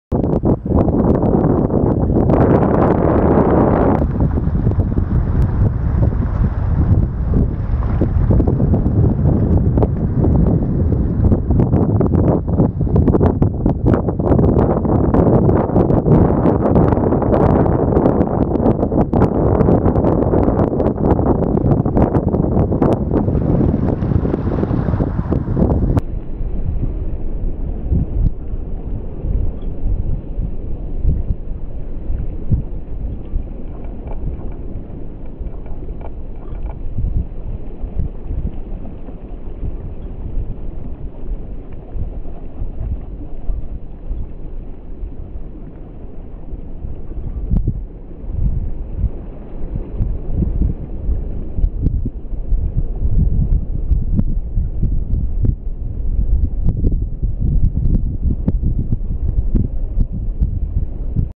Бухта Балос — одно из самых красивых мест мира, о. Крит, Греция
Утро в бухте Балос - это одна из достопримечательностей Крита и Греции, место, где произошло слияние трёх морей, здесь встретились Критское, Средиземное и Ионическое море.